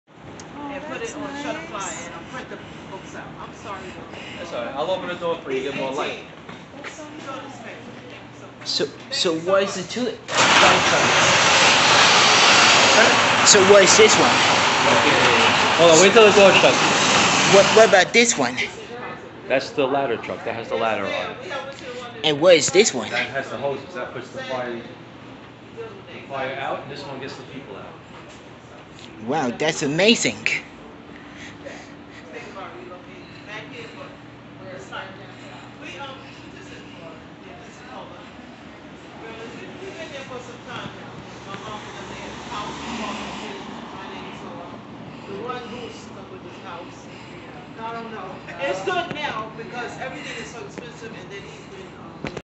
Fleet Saturday Firehouse Tour Episode 11 FDNY 2005 Tiller Ladder 118 and FDNY Engine 205 KME 2016
Welcome to Fleet Saturday Today Were Gonna Visit to The Henry St Cadman Plaza Brooklyn FDNY 2005 seagrave Tiller Ladder 118 Formed By Ex Ladder 104 pride of the Williamsburg